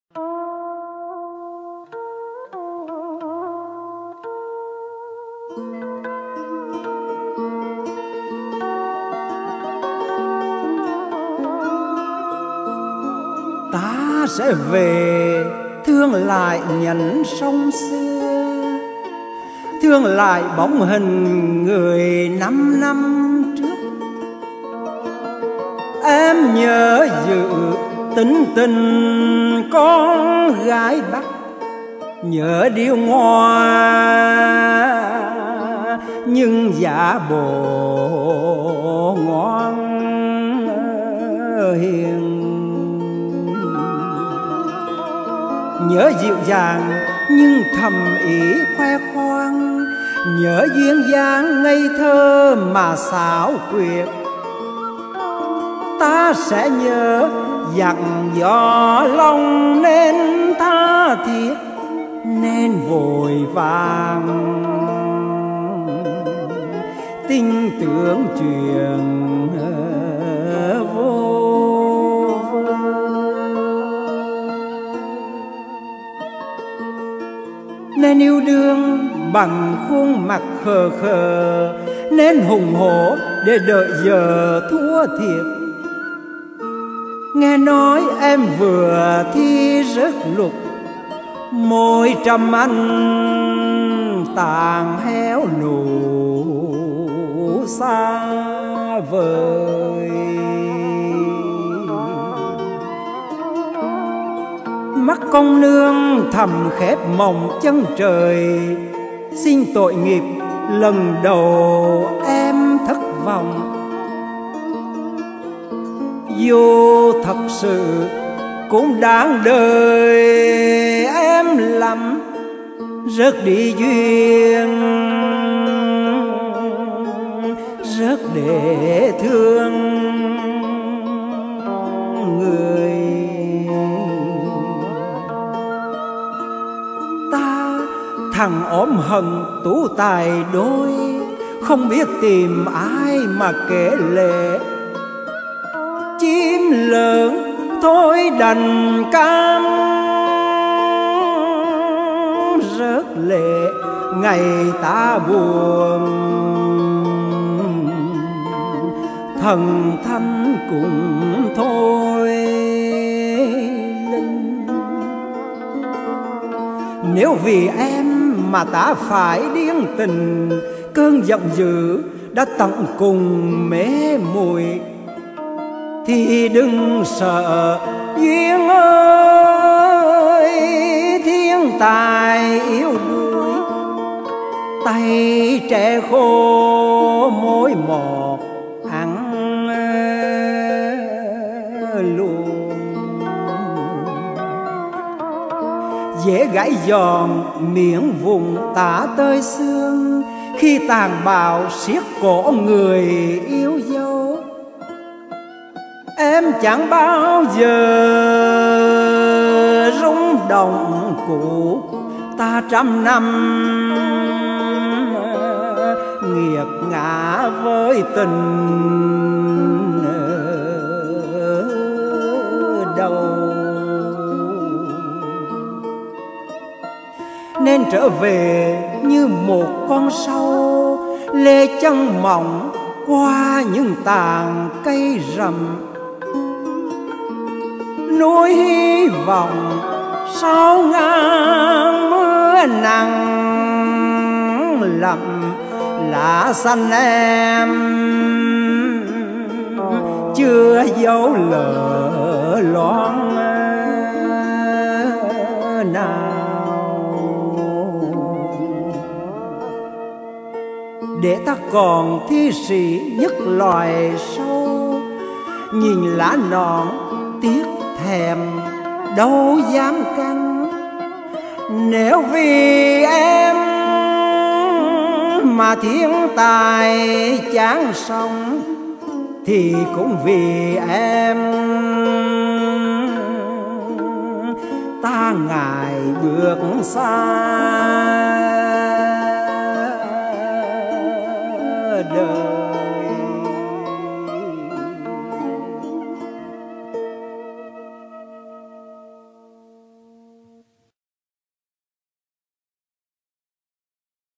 Duyên tình con gái Bắc-Nguyễn Tất Nhiên-Ngọc Sang ngâm thơ
Ta sẽ về thương lại nhánh sông xưa
thương lại bóng hình người năm năm trước...
em nhớ giữ tính tình con gái Bắc
nhớ điêu ngoa nhưng giả bộ ngoan hiền
nhớ dịu dàng nhưng thâm ý khoe khoang
nhớ duyên dáng, ngây thơ... mà xảo quyệt!

ta sẽ nhớ dặn dò lòng nên tha thiết
nên vội vàng tin tưởng chuyện vu vơ
nên yêu đương bằng khuôn mặt khờ khờ
nên hùng hổ... để đợi giờ thua thiệt!

nghe nói em vừa thi rớt Luật
môi trâm anh tàn héo nụ-xa-vời
mắt công nương thầm khép mộng chân trời
xin tội nghiệp lần đầu em thất vọng!
(dù thật sự cũng đáng đời em lắm
rớt đi Duyên, rớt để thương người!)

ta - thằng ôm hận tú tài đôi
không biết tìm ai mà kể lể
chim lớn thôi đành cam rớt lệ
ngày ta buồn thần thánh cũng thôi linh!

nếu vì em mà ta phải điên tình
cơn giận dữ đã tận cùng mê muội
thì đừng sợ, Duyên ơi, thiên tài yếu đuối
tay tre khô mối mọt ăn luồn
dễ gãy dòn miểng vụn tả tơi xương
khi tàn bạo siết cổ người yêu dấu!

em chẳng bao giờ rung động cũ
ta năm năm nghiệt ngã với tình đầu
nên trở về như một con sâu
lê chân mỏng qua những tàn cây rậm
nuôi hy vọng sau ngàn mưa nắng lậm
lá-xanh-em chưa dấu lở loang nào
để ta còn thi sĩ nhất loài sâu
nhìn lá nõn, tiếc, thèm... đâu dám cắn!

nếu vì em mà thiên tài chán sống
thì cũng vì em ta ngại bước xa đời!